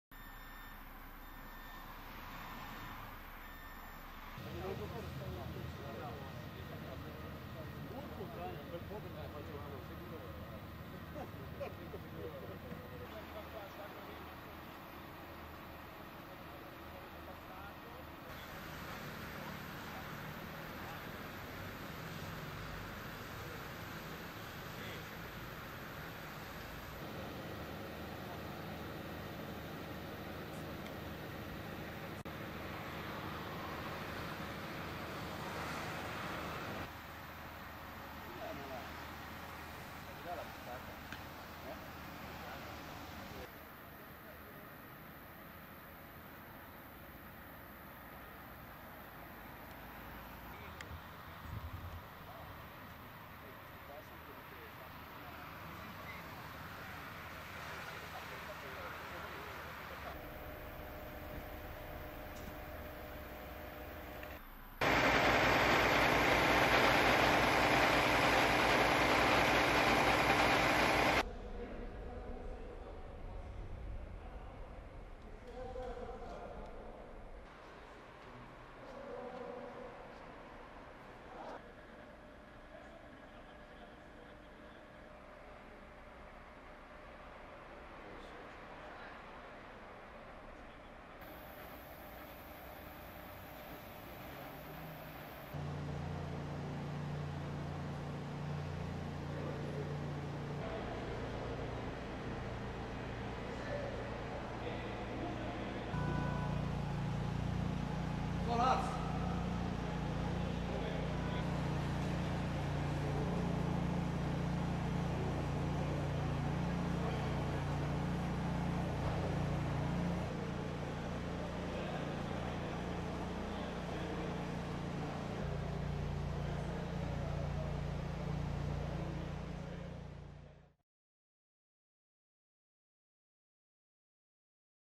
Incidente A2 Grancia
Incidente sull'A2 nella galleria di Grancia dove auto si rovescia e striscia per 200 metri.